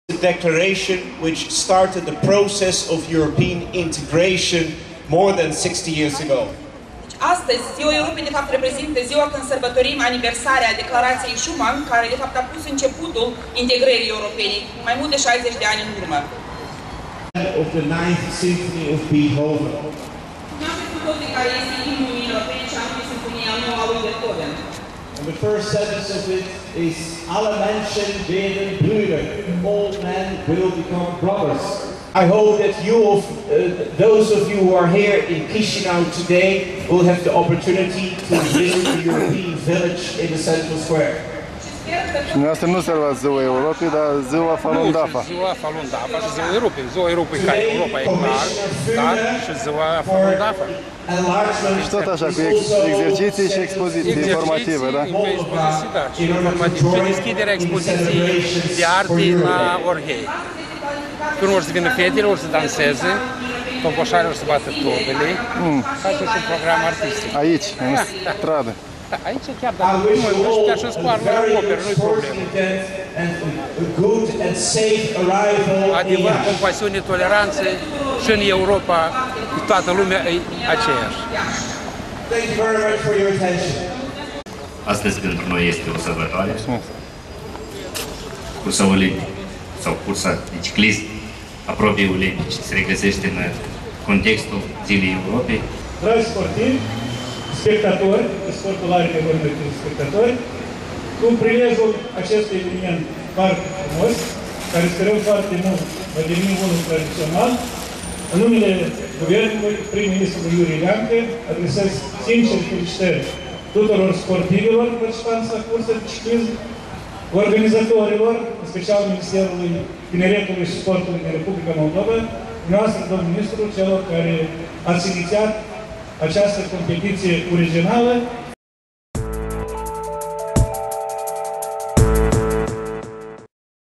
Falun Dafa serba în Scuarul Europei
Un reprezentant al asociaţiei Falun Gong Moldova pregătea terenul pentru manifestaţia tradiţională de sîmbătă dimineala, la intrare în parc, dar, spune el, azi e o sărbătoare deosebită, ziua internaţională Falun Dafa.
În aceeaşi zi era marcată la Chişinău Ziua Europei şi se dădea startul unei curse ciclistice spre Iaşi.